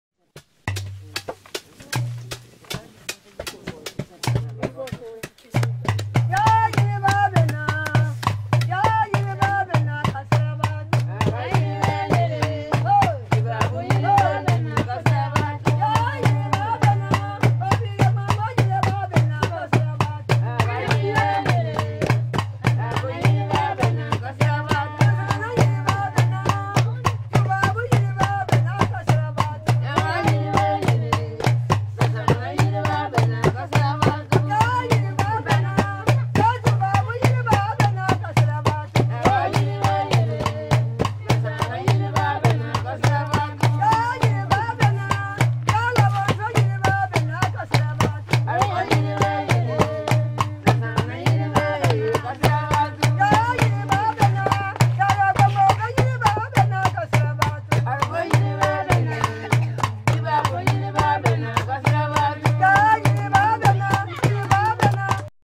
mortar pounding 3 part rhythm in Burkina Faso with water (calabash) drums
a completely different type of "cancel culture," with connections to the work rhythms of these West African women, as well...